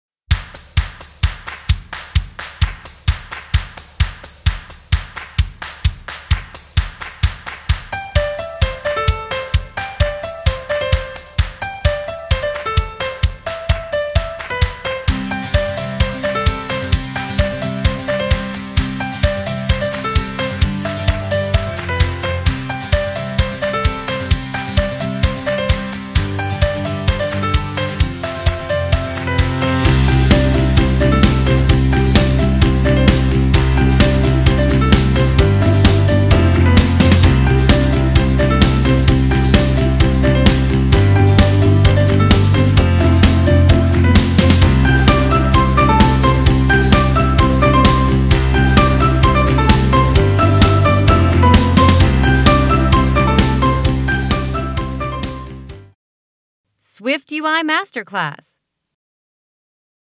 Learn advanced gestures in iOS app development with this SwiftUI tutorial
1. SwiftData Masterclass ♥️

2. SwiftUI Masterclass ♥️

3. Credo Academy ♥️ WELCOME25

. Learn advanced gestures in iOS app development with this SwiftUI tutorial using Swift programming language in the Xcode code editor.